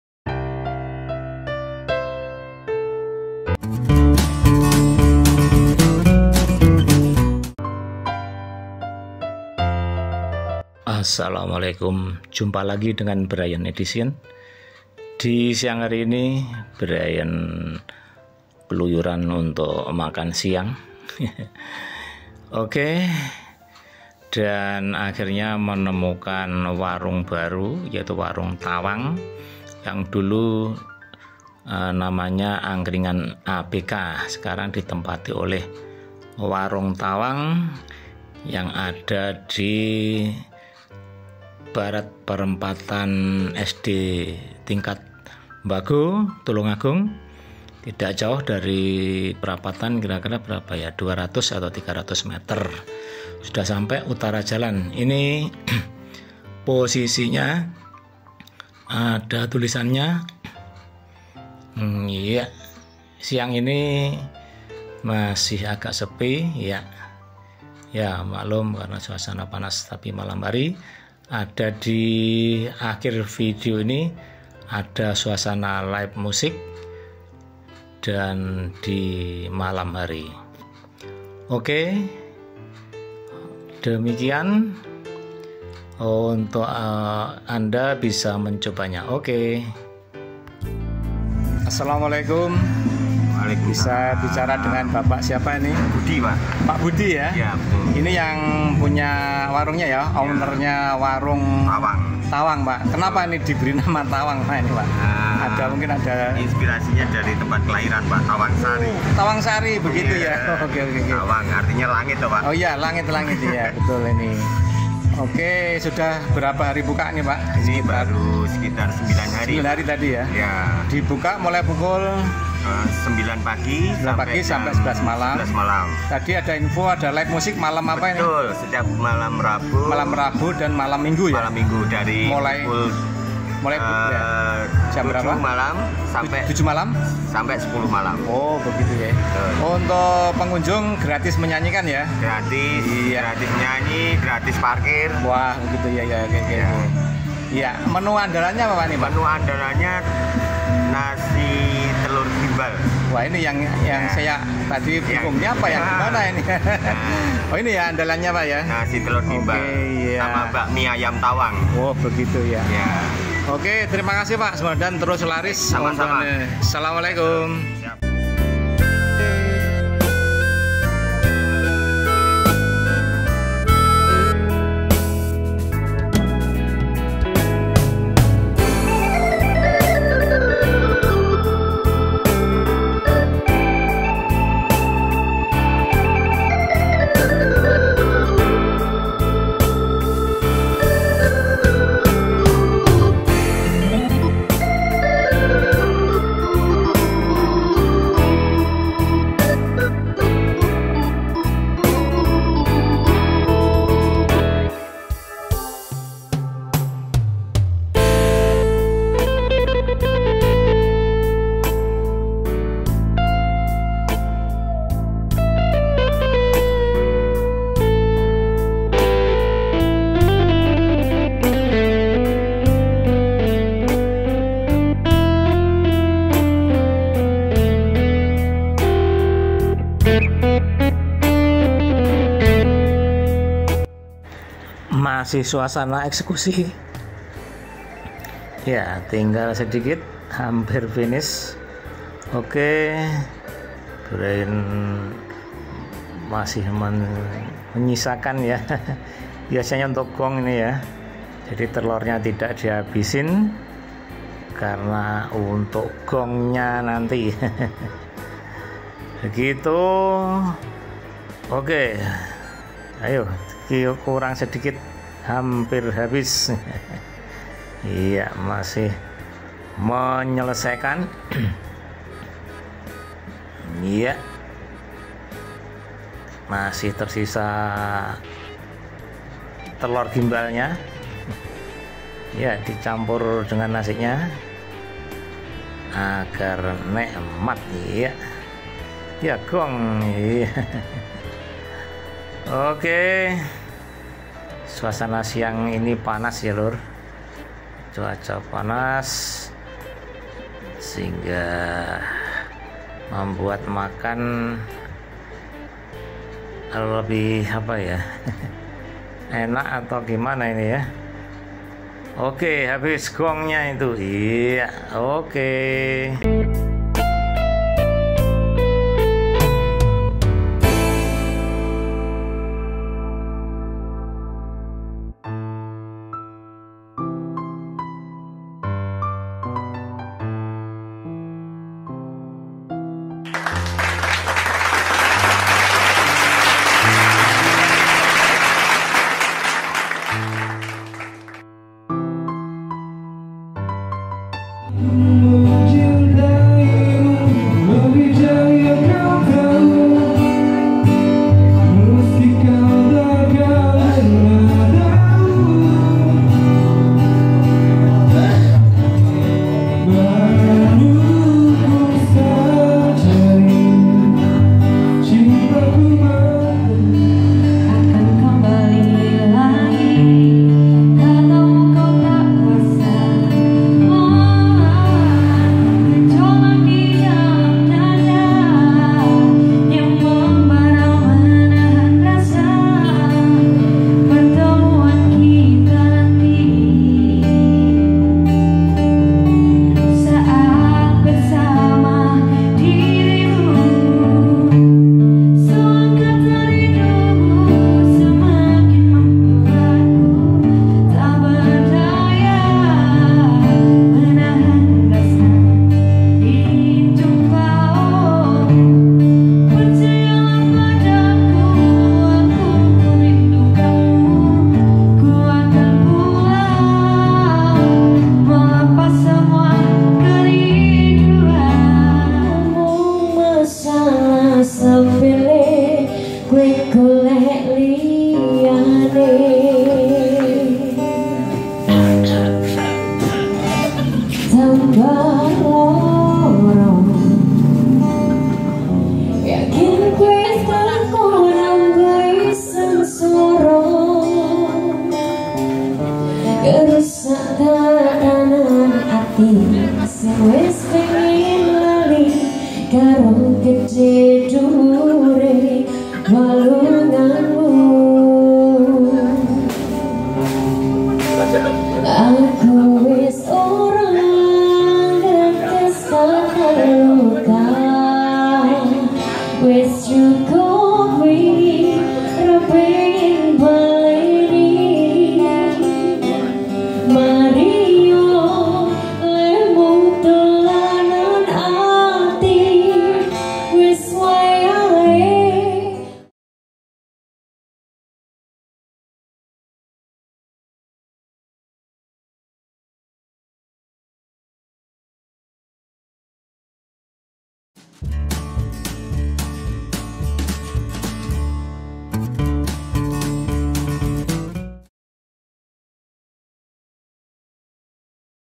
VIRAL KAN NEW Warung Tawang Special Nasi Telor Gimbal & Bakmi Ayam Tawang Bago Tulungagung East Java
@brianthadi Edition masih mendokumentasikan Kuliner yang ada di Tulungagung & sekitarnya. Kali ini di Warung Tawang, Bago Tulungagung East Java Indonesia. Bagaimana reviewnya? simak saja videonya hingga selesai, Terima kasih 🙏🙏🙏 info terkini sudah tutup.. 🙏